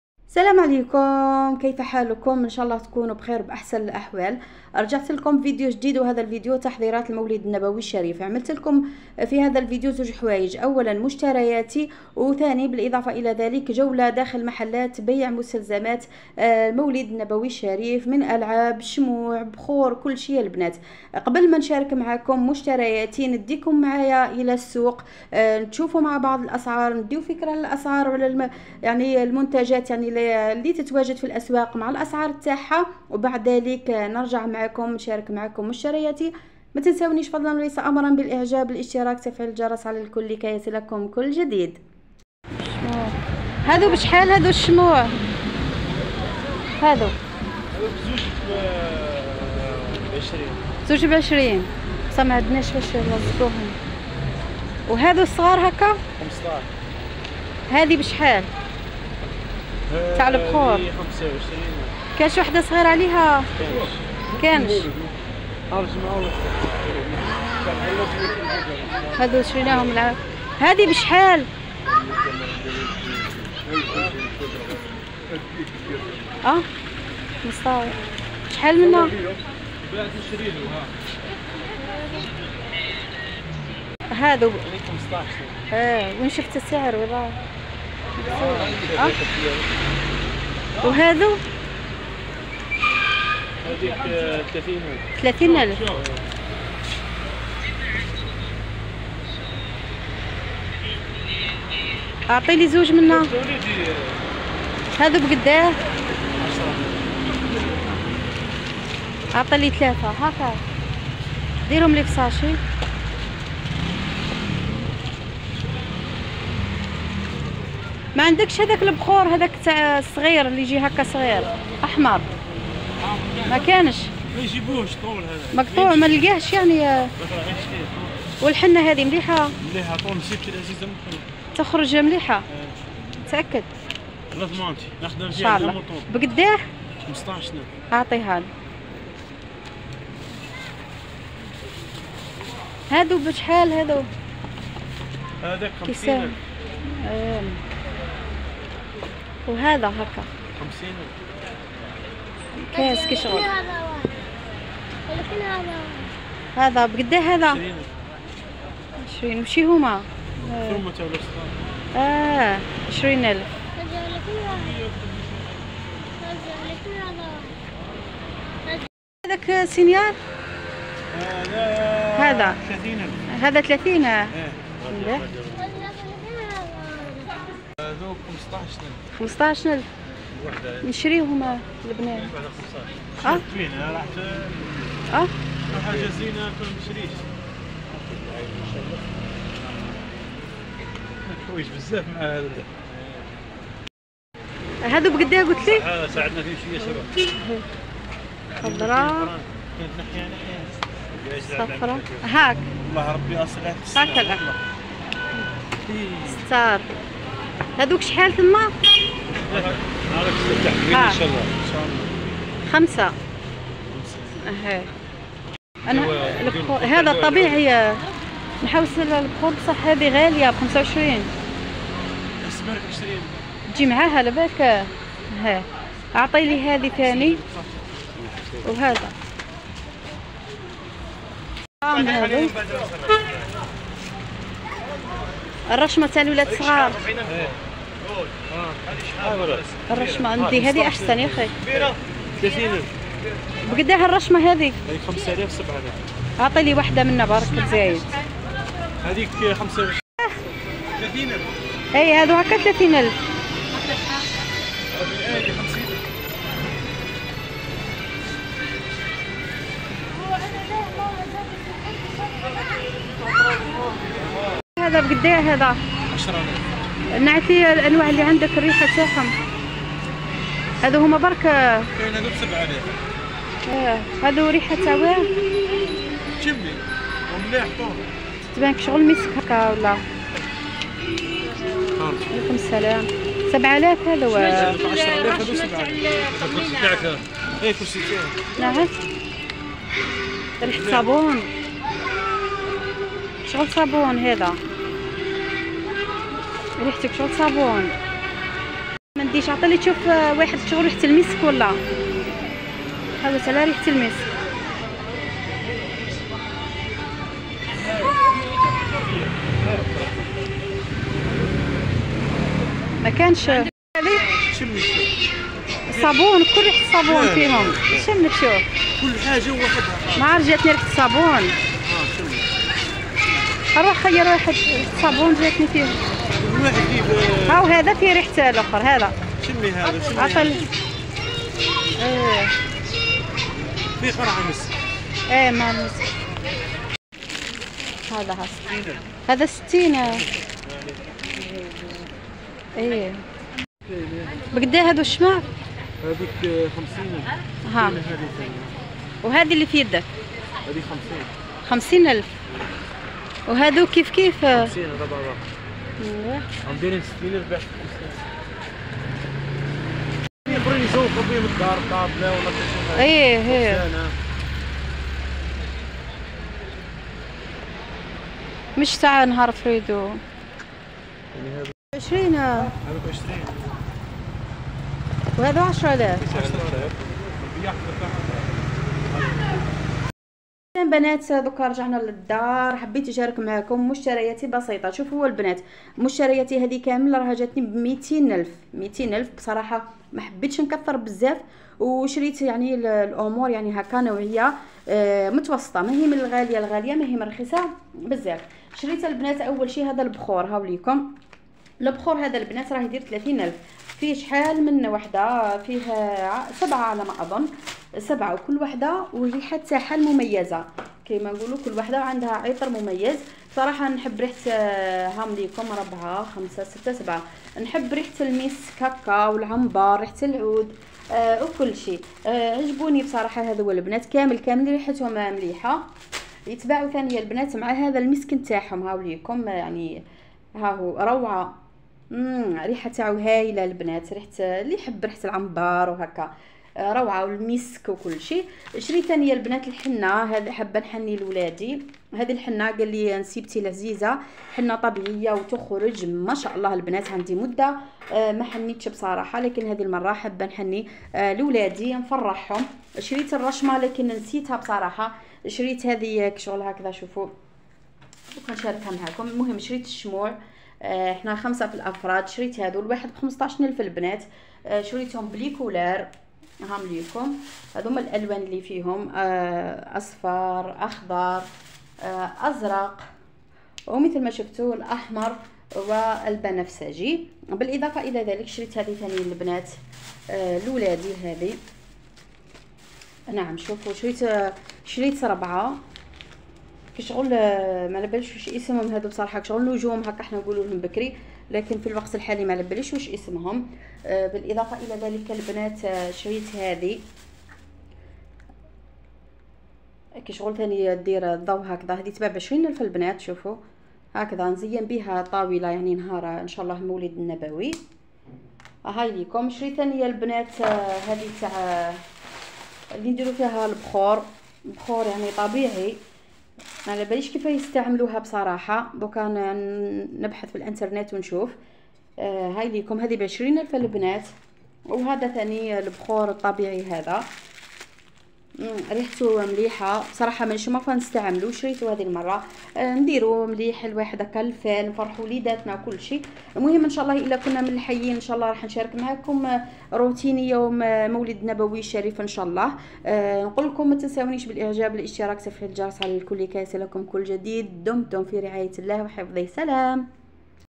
فرحت وليداتي بأقل التكاليف/ مشترياتي للمولد النبوي 2022 / جولة في سوق بيع ألعاب المولد النبوي
تحضيرات المولد النبوي الشريف 2022
مشتريات الاحتفال بالمولد النبوي الشريف مع الاسعار
المولد النبوي الشريف 2022
أجواء الاحتفال بالمولد النبوي الشريف 2022